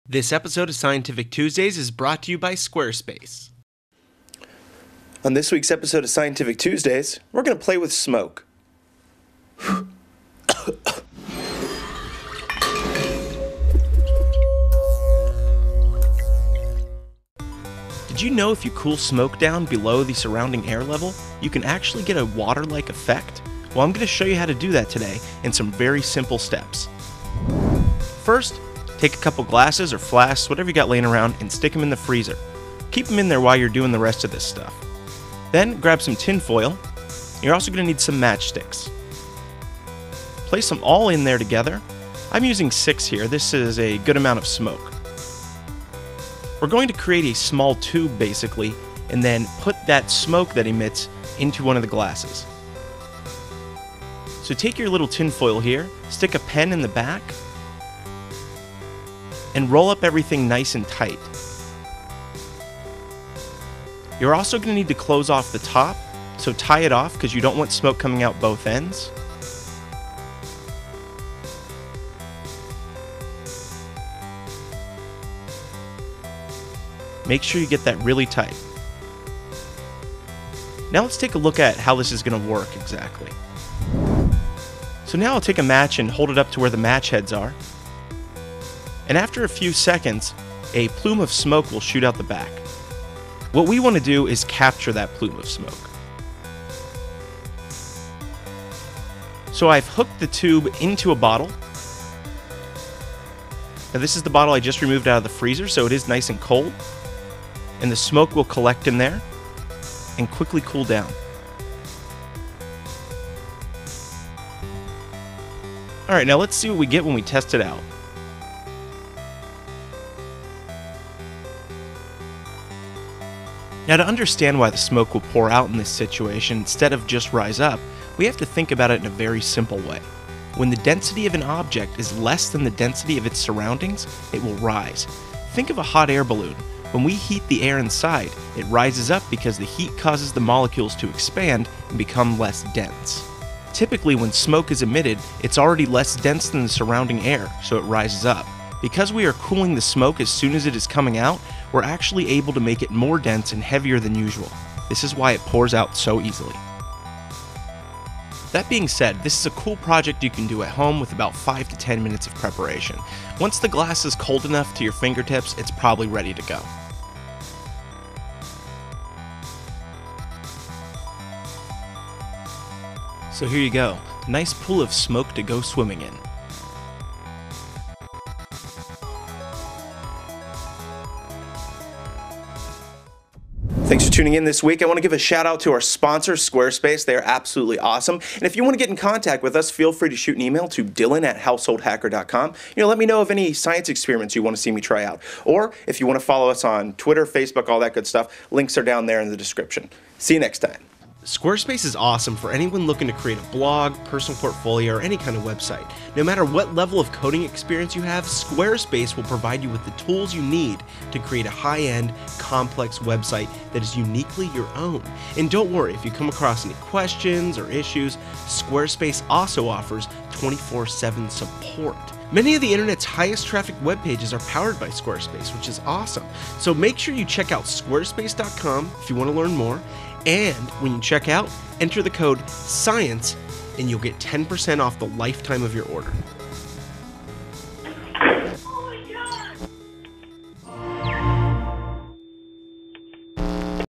Scientific Tuesdays - Sinking Smoke (no cigarette needed)
New Science Tricks weekly!

Heat rises and smoke is usually hotter than the air surrounding it. So how can we make it colder and force it to sink like a rock? In this episode we will take you through the easy to follow steps involved in turning smoke into a liquid-like state.

Did you enjoy the music?